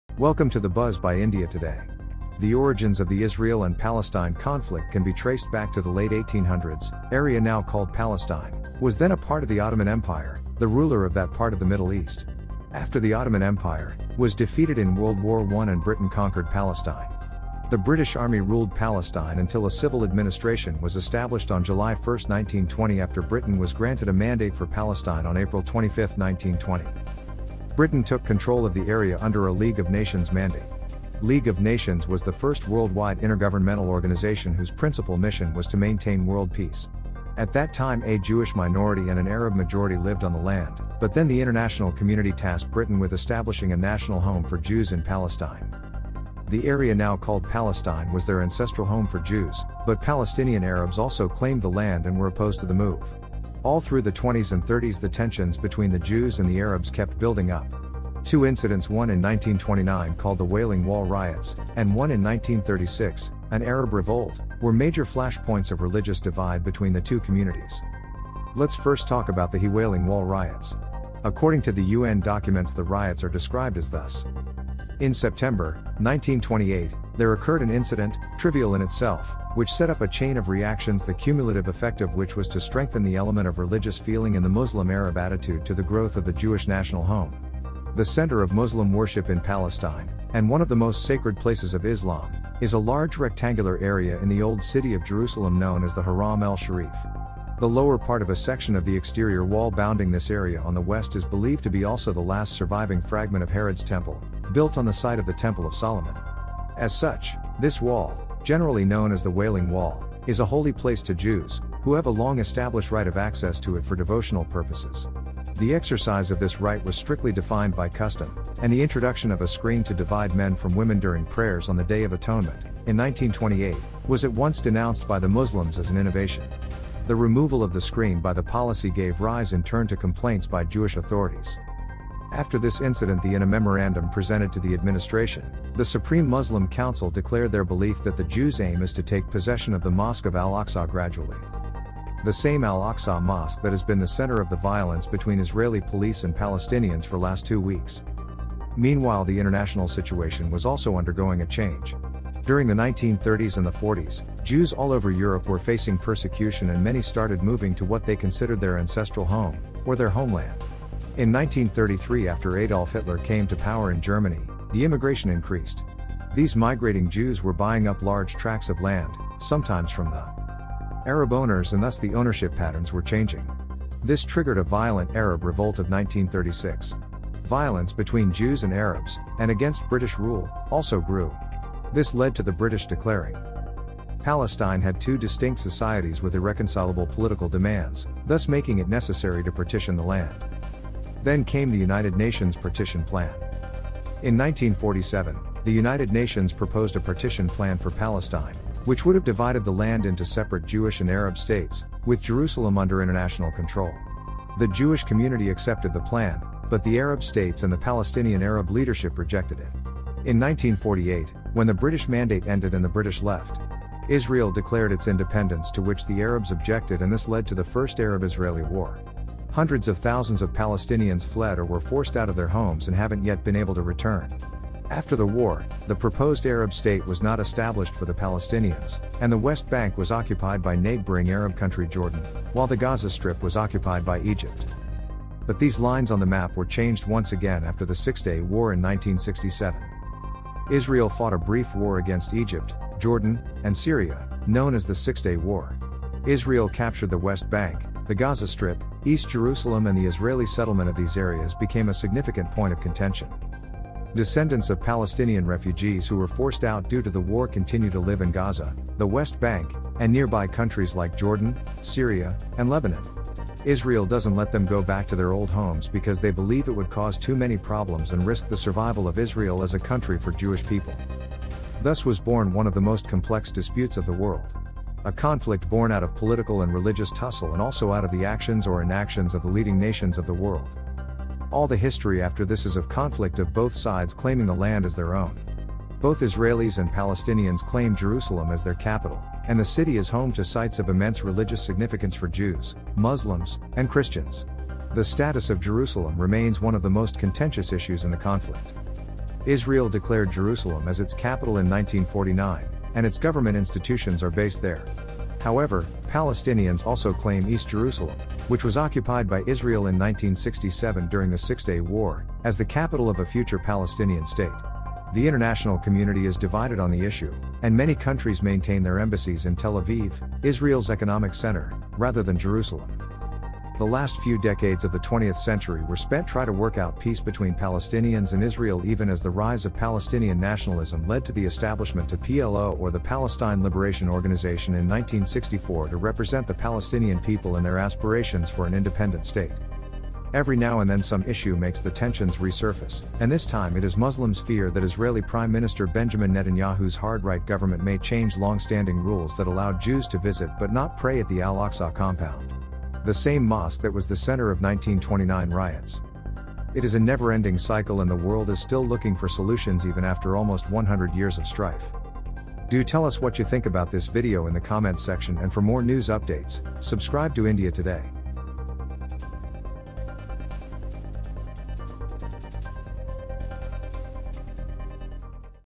Israel-Palestine Conflict Explained: Why Are Israel & Palestine At War? BUZZ Around The Globe
Israel-Palestine conflict intensifies as more than 800 targets have been struck as Israel intensified bombardments of the Gaza Strip to crush Hamas fighters.

Hamas’ unprecedented incursion and Israel’s retaliation have killed more than 1,100 people and wounded thousands on both sides.

Israel’s declaration of war has portended greater fighting ahead.

And a major question is whether Israel will launch a ground assault into Gaza, a move that in the past has brought more casualties.